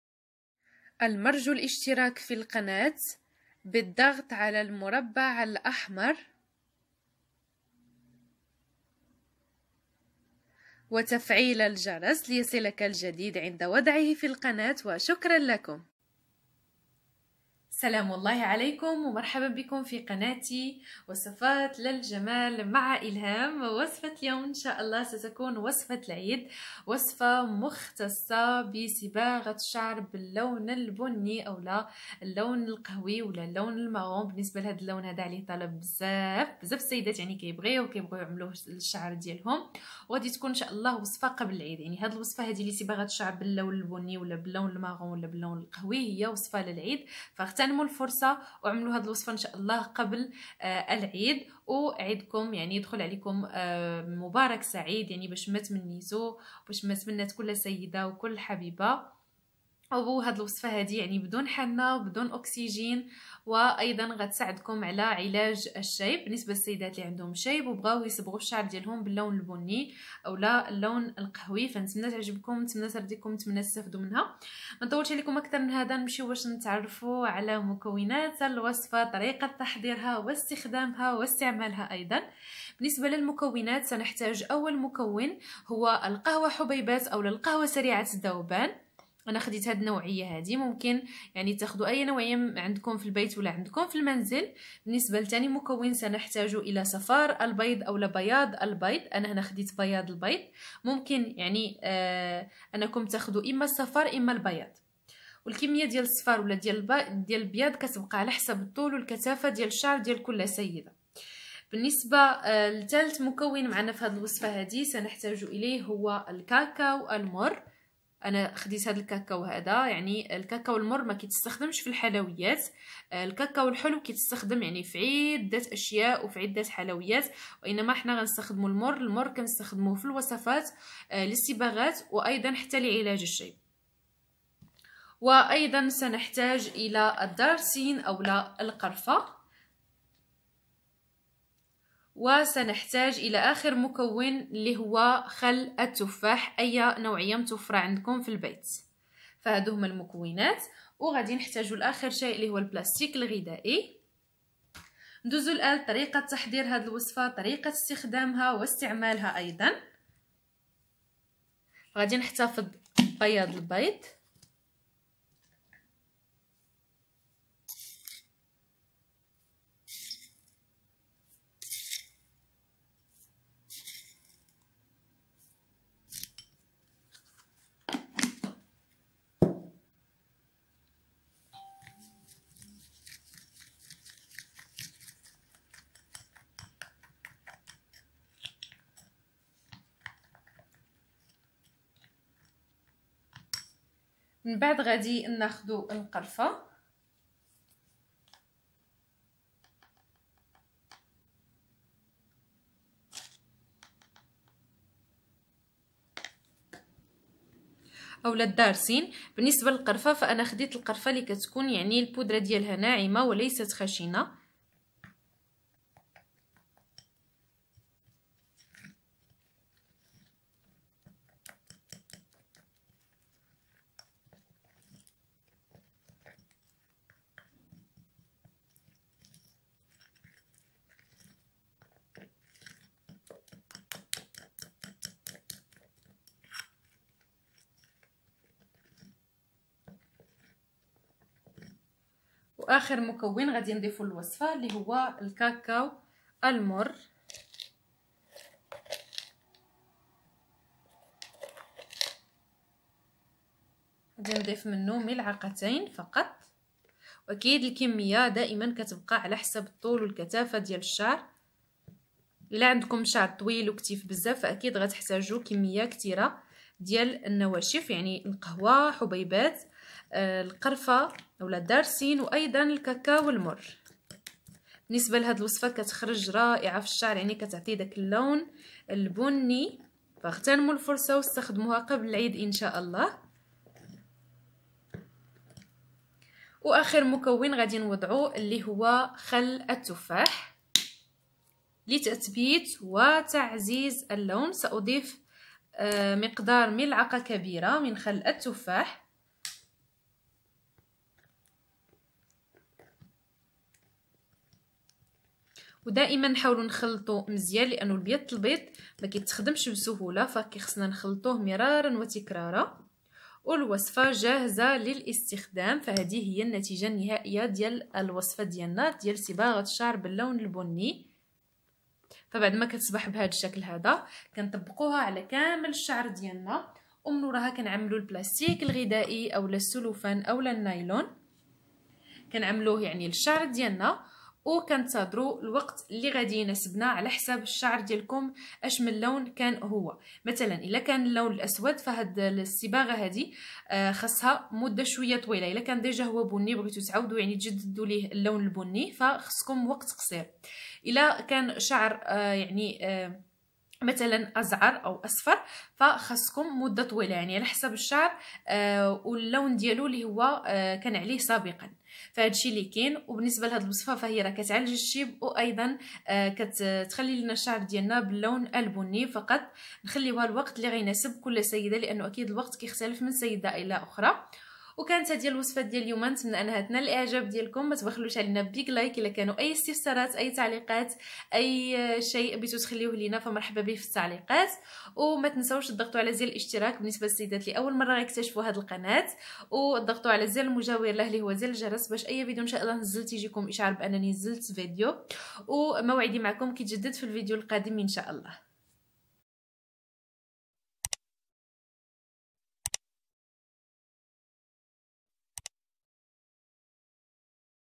اصبغي شعرك للعيد لون بني | بمكونات طبيعية بدون حناء ولا اوكسجين وبدون شيب والنتيجة مذهلة مجربة!
✥رجاءً لا تنسوا تشتركوا بالقناة